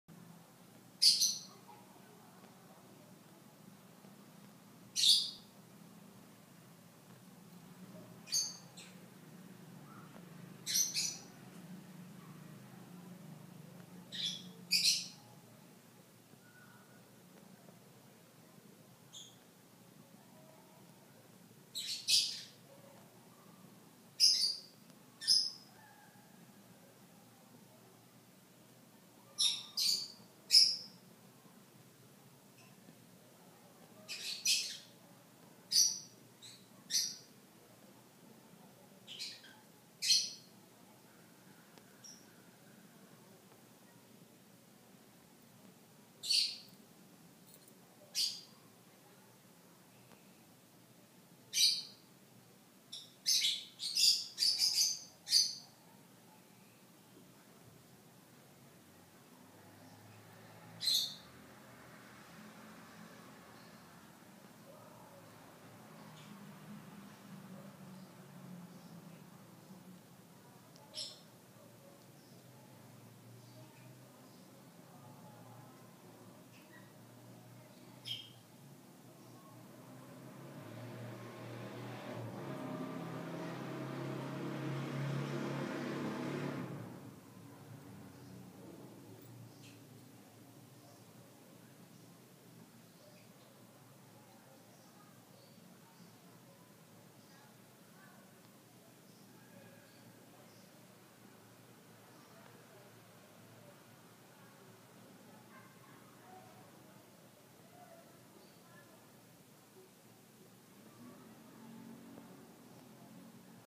Mulán y el canto de los pájaros
Mulán con su cobija favorita y  el cantar de lo pájaros acechados diariamente.